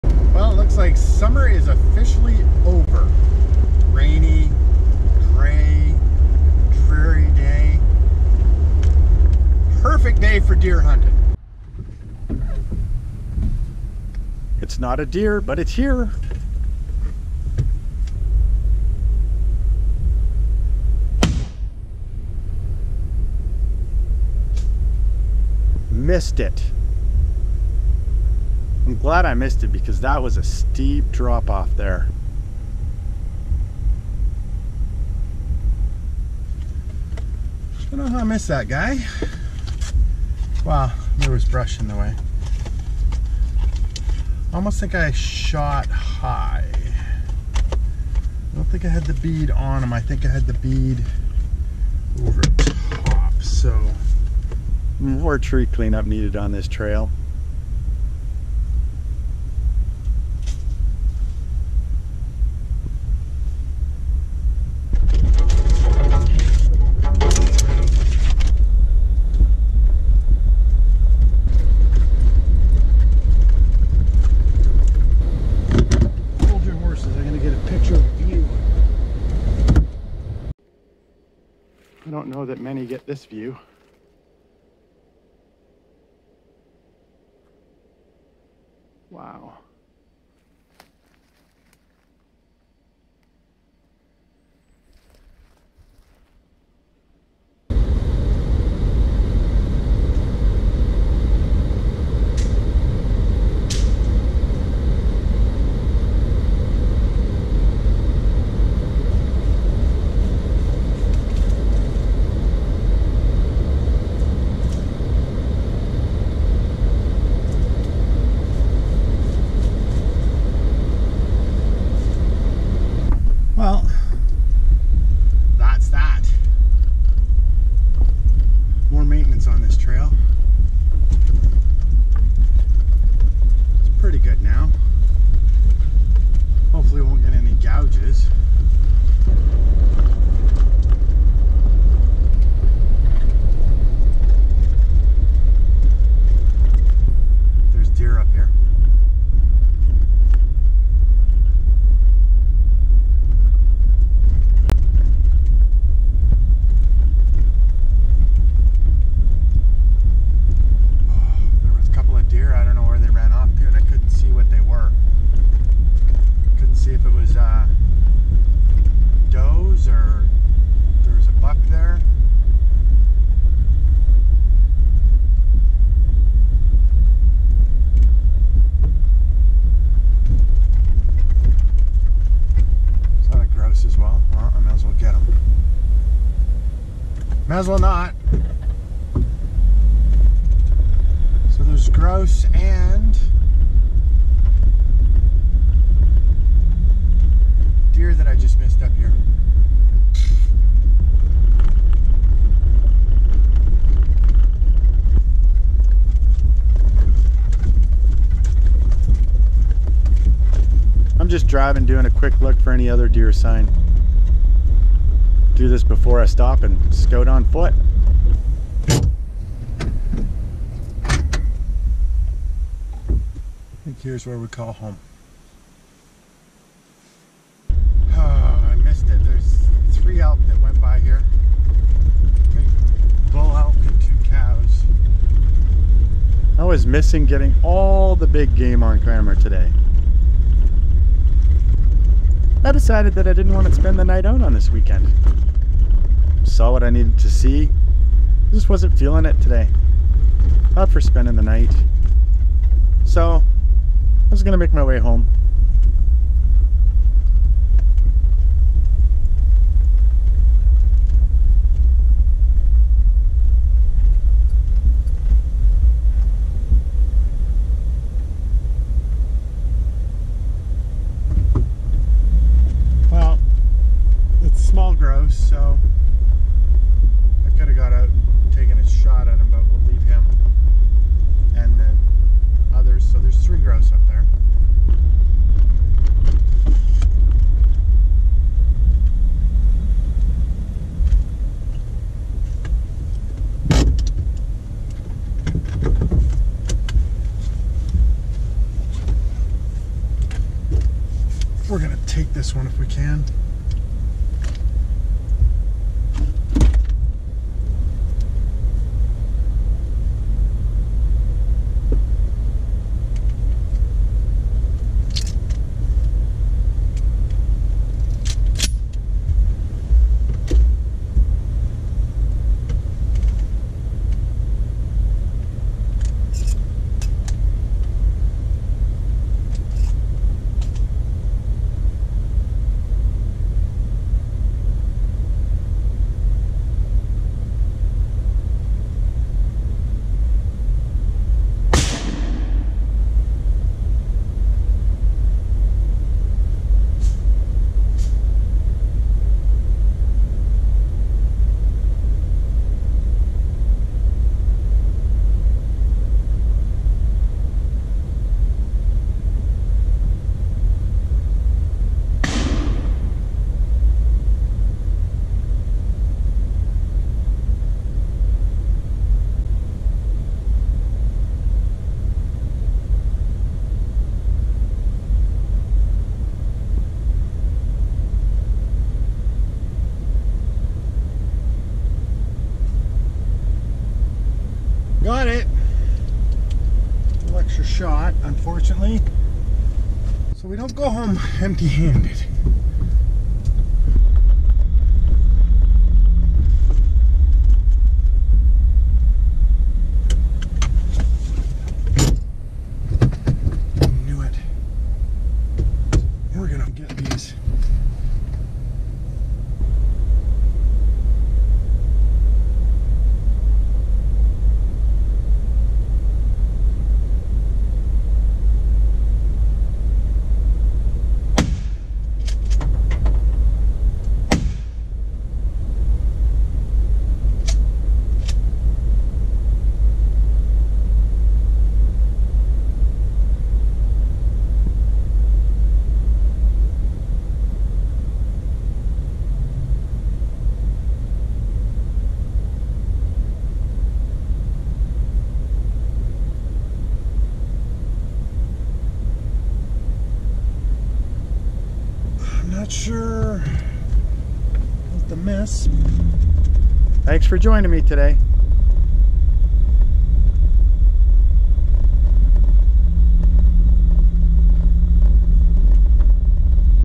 Black-Tailed Deer Hunting - Missed Opportunity
The plan for the day was to head out to the mountains, get to my spot, and do some scouting for deer.

Before I got to my spot, I wanted to check the newly cleared trail that I worked on a couple of weeks previous. I was  able to see a grouse and I also had more tree clearing to do.

During the day, I saw 2 black-tailed deer, 5 elk, and 6 grouse. I ended up coming home with 2 grouse for the day.

I  missed an opportunity on deer, but I was  happy to see them. That means I'm in the right area :-)